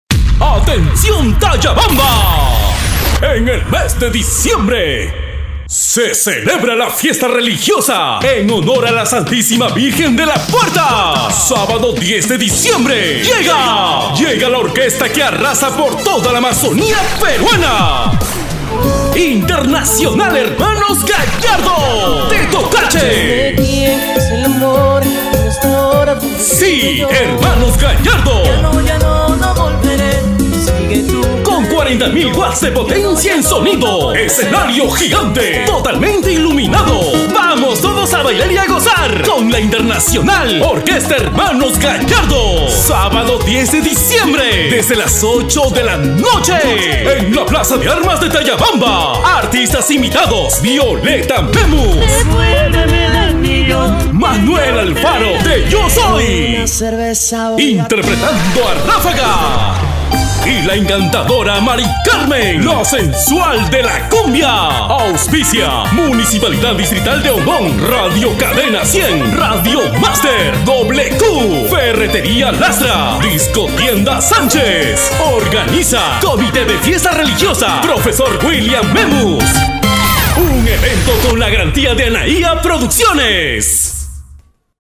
TAYABAMBA SPOT FIESTA VIRGEN DE LA PUERTA TAYABAMBA 2016
FIESTA PATRONAL EN HONOR A LA VIRGEN DE LA PUERTA  TAYABAMBA 2016. LLEGA DESDE TOCACHE  ORQUESTA HERMANOS GALLARDO.